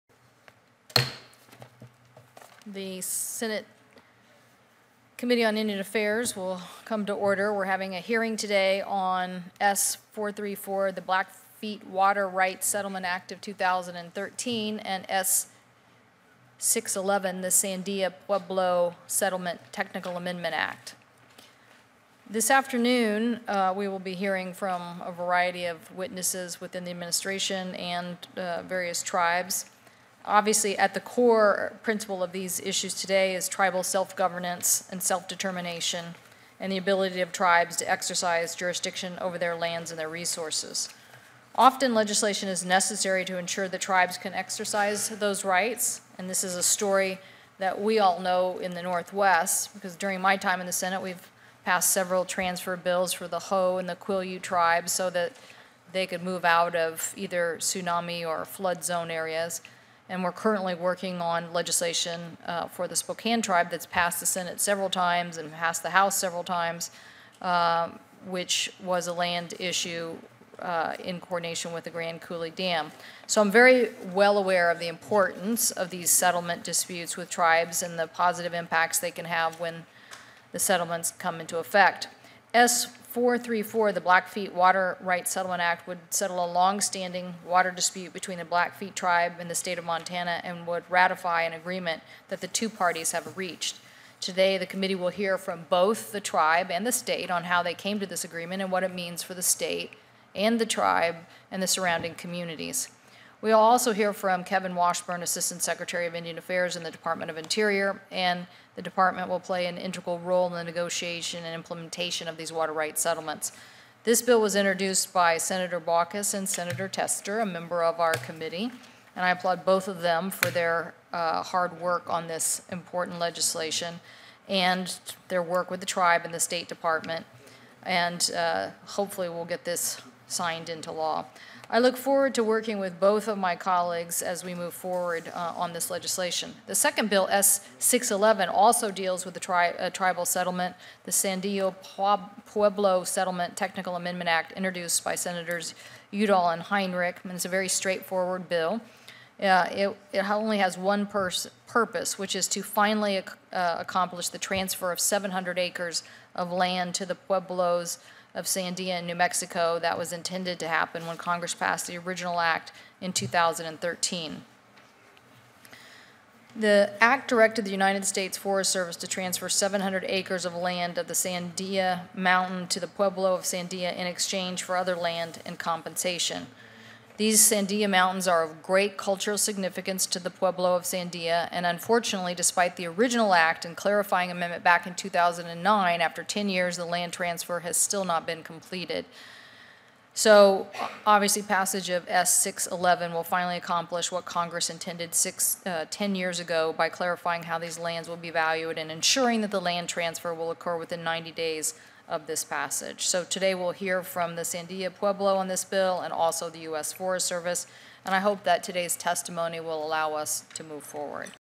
Chairwoman Cantwell Holds Hearing on Tribal Resources Legislation, Opening Statement
On May 8, 2013, Chairwoman Maria Cantwell (D-WA) held a legislative hearing to address water and lands rights that are essential to the Blackfeet Nation of Browning, Montana, and the Sandia Pueblo of Bernalillo, New Mexico. The hearing examined the Blackfeet Water Rights Settlement Act of 2013 (S. 434) and the Sandia Pueblo Settlement Technical Amendment Act (S. 611).